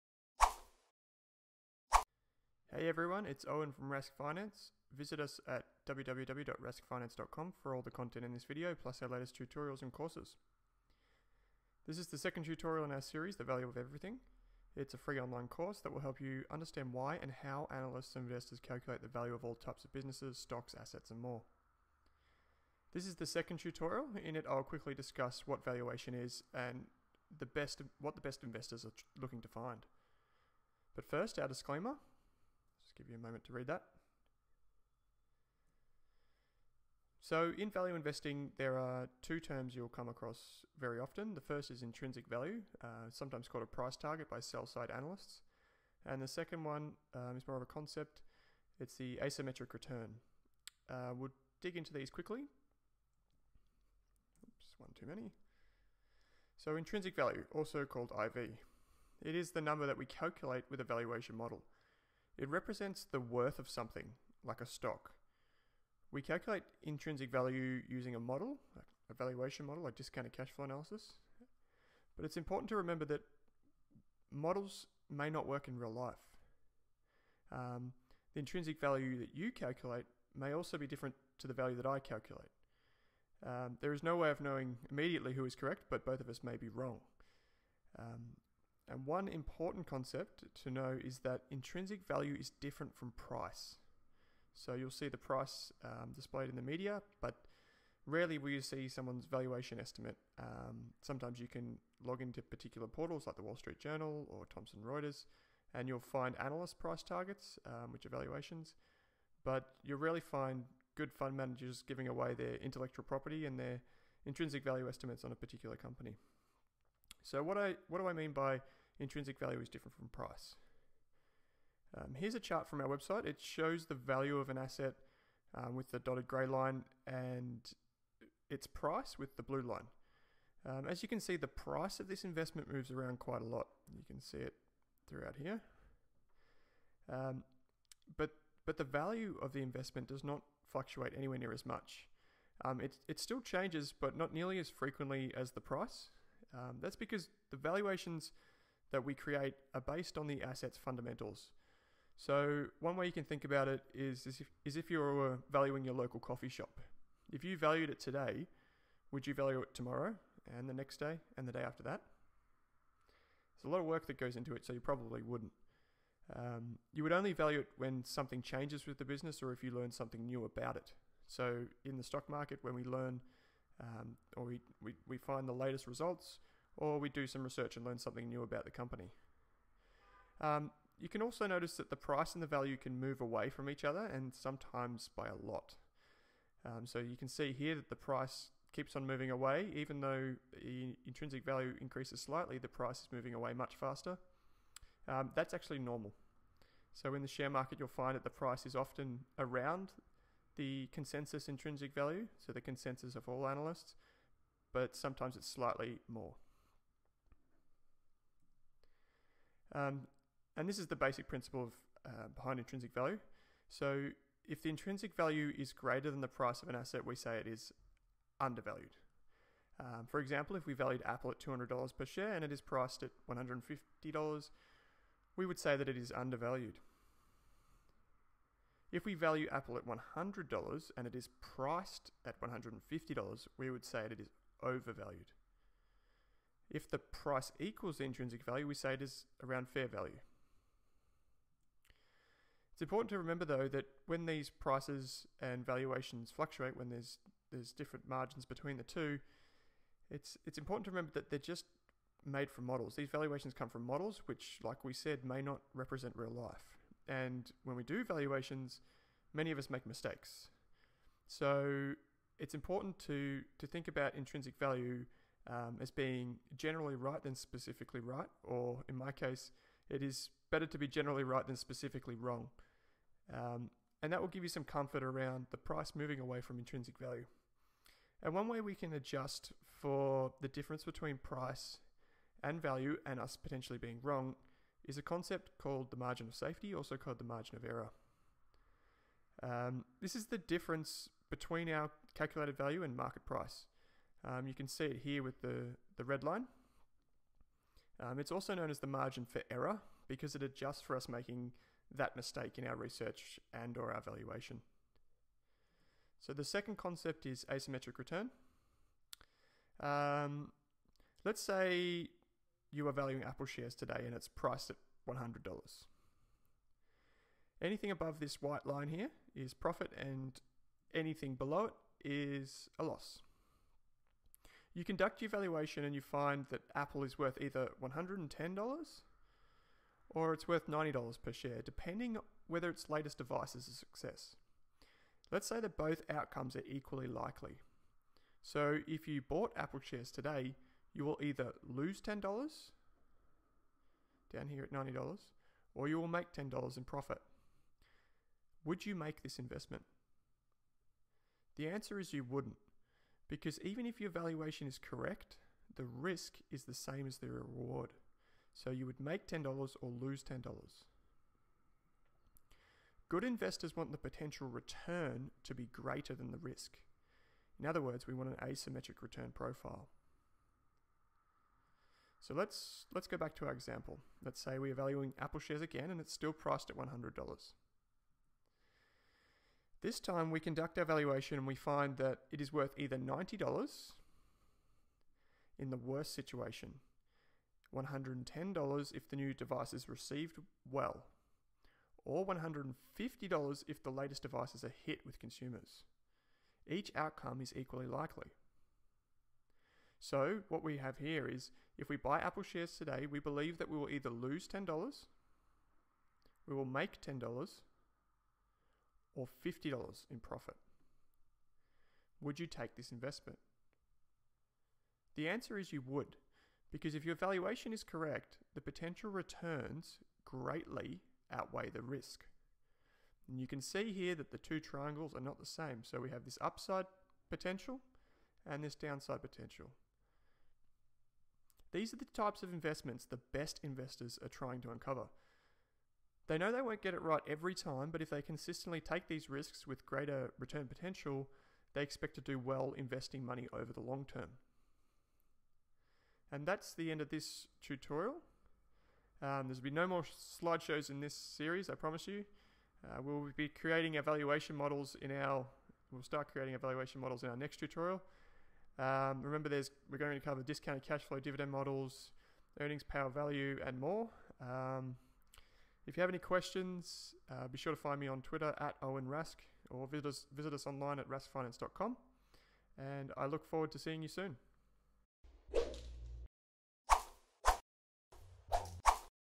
The Purpose of Valuation | The Value of Everything | Rask Finance | [HD]
The "Value of Everything" is our comprehensive valuation course.
We cover:
- Discounted Cash Flow (DCF) analysis
- Dividend Discount Models (DDM)
- Earnings Power Value (EPV) and more!

We use a REAL LIFE EXAMPLE.

to find this course and many more. It's FREE.